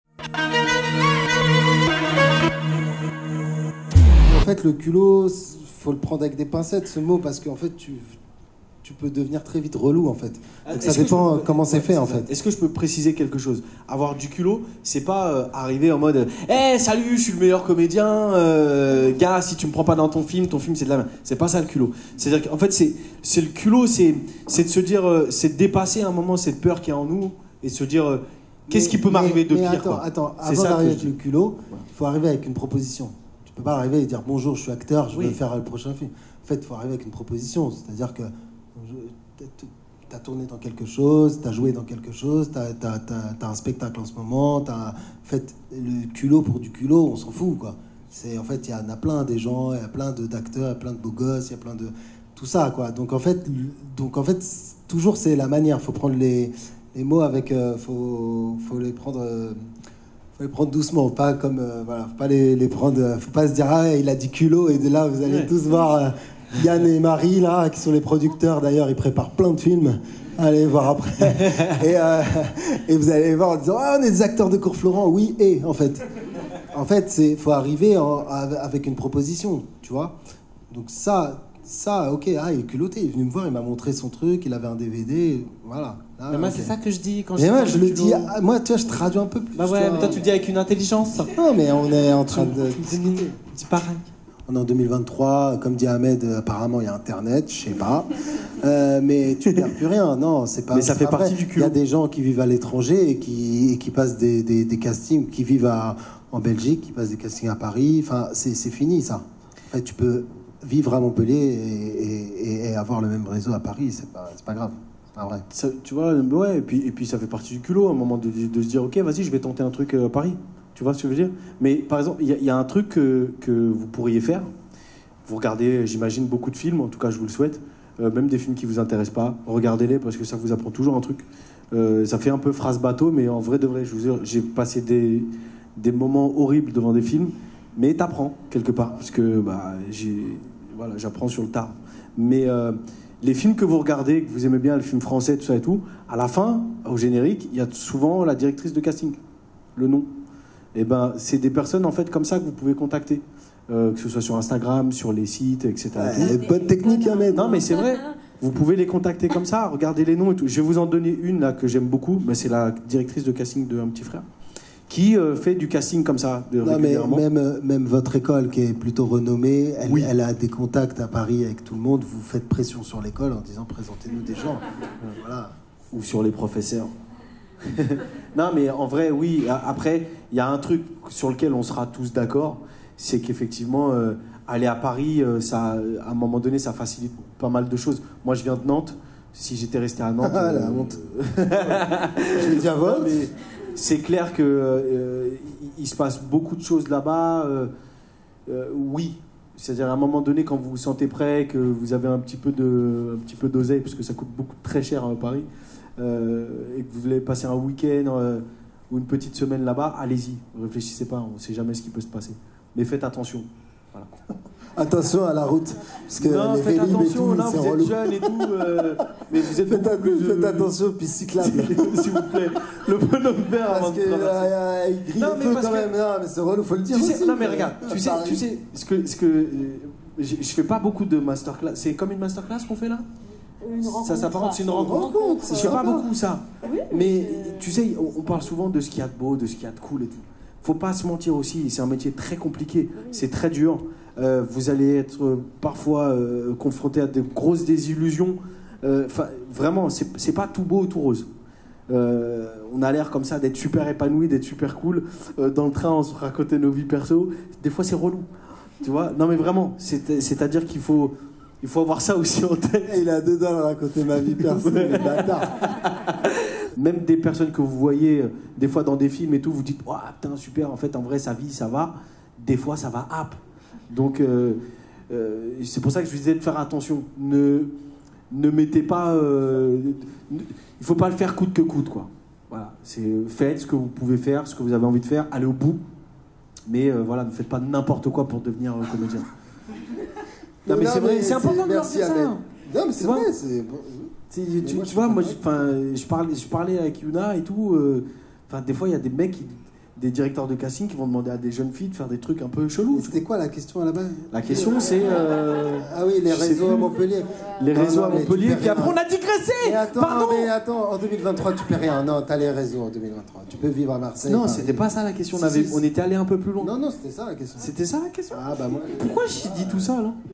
45ème CINÉMED : Ali Marhyar & Ahmed Sylla (Rencontres avec les élèves du cours Florent Montpellier)
Dans le cadre des échanges avec les écoles de cinéma de la ville de Montpellier, les élèves inscrits rencontrent les acteurs et réalisateurs invités au festival de cinéma !